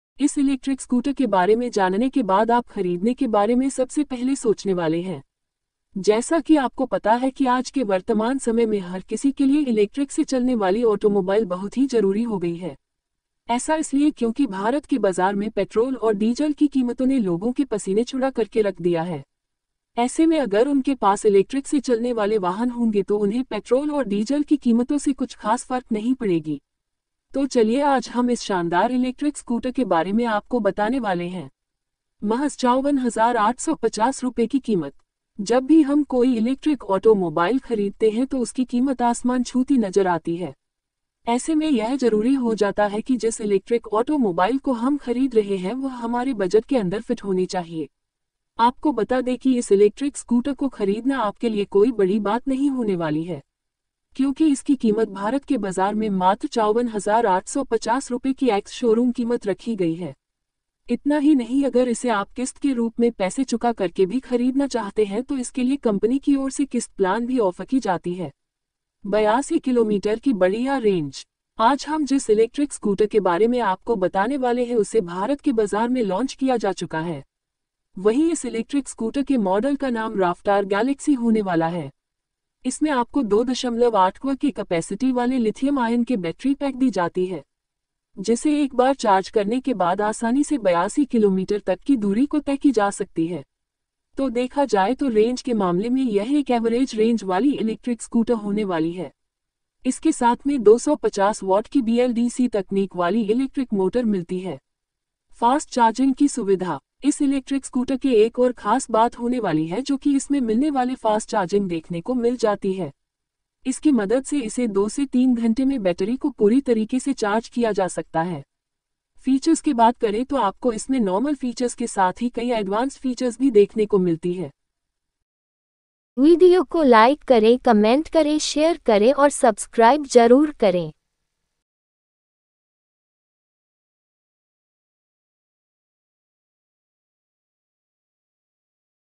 Raftaar Galaxy Electric Scooter: 100 किलोमीटर रेंज 😱वाला EV स्कूटर मिलेगा सिर्फ 51 हजार रुपये में..!
भारत में रफ़्तार स्कूटर प्राइस लिस्ट 2024
मॉडल एक्स-शोरूम कीमत माइलेज / रेंज
रफ़्तार गैलेक्सी ₹. 51,900 100 की.मी./चार्ज
रफ़्तार इलेक्ट्रिका. ₹. 48,540 - 70,900 100 की.मी./चार्ज
रफ्तार बुमबलेबी  ₹. 88,000 100 की.मी./चार्ज
रफ्तार क्रूजर आर1 ₹. 84,500 100 की.मी./चार्ज

– Key Specs & Features of Galaxy

- Range : 100 km/charge
- Motor Power : 250 W
- Motor : BLDC
- Brakes : Double Disc
- Tyre Type : Tubeless
- Charging Time : 4-5 hours

About Raftaar Galaxy
The Raftaar Galaxy is an electric scooter, available in 1 variant, at a starting price of Rs 51,900.The Galaxy comes with a Disc brake up front and Disc brake at the rear. You can buy the Galaxy in 4 colours - Black, Red, Blue, Gray.

Raftaar Galaxy Price
The price of Raftaar Galaxy in India starts at Rs. 51,900 Raftaar Galaxy is offered in 1 variant - Raftaar Galaxy STD which comes at a price tag of Rs. 51,900.